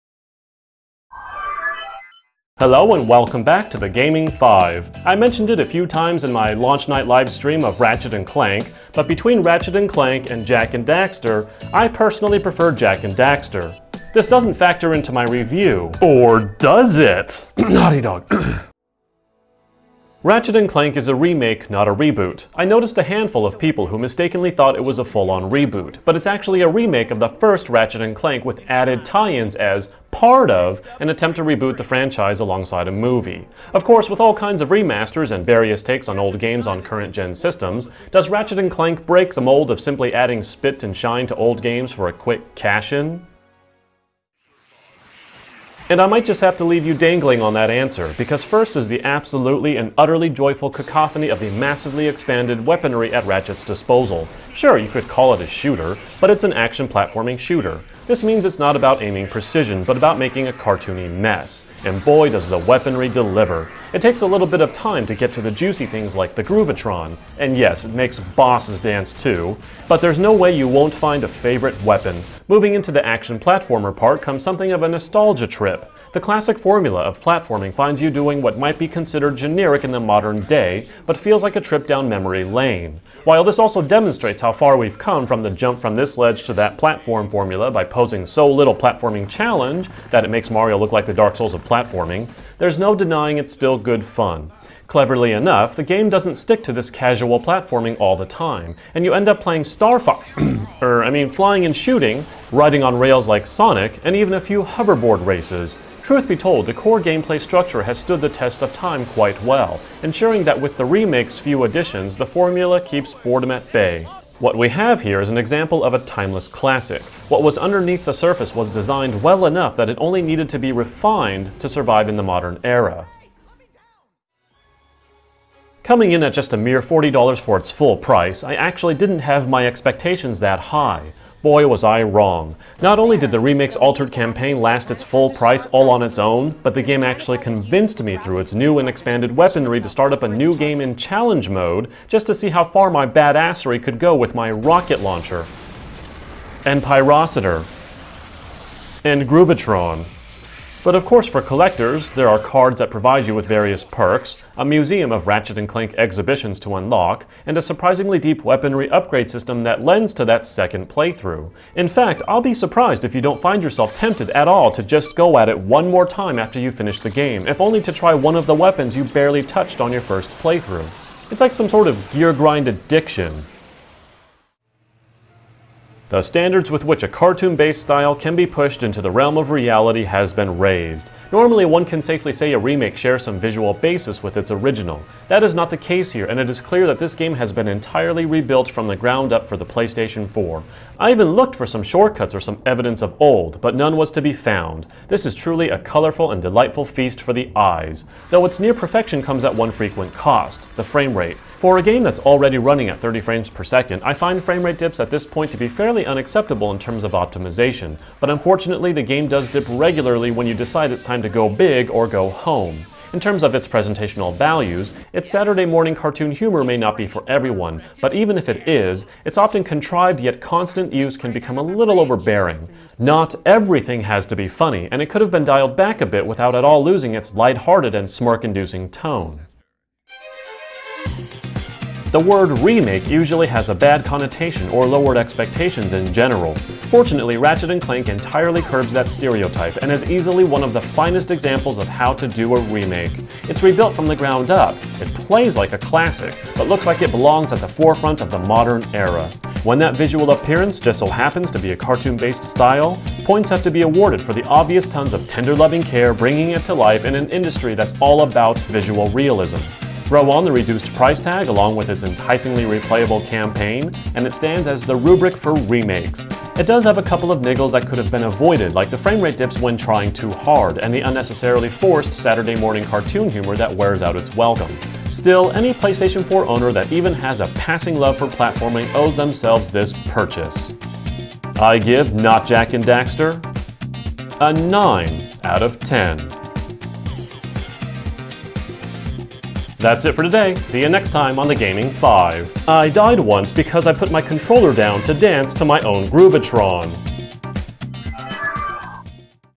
Ratchet & Clank (Review of Not the Movie!)
Yes, this is my review of not the movie based on the game that's not based on the movie but has tie-ins based on the movie.

Miiverse: ZyroXZ2
XBL: ZyroXZ2
PSN: ZyroXZ2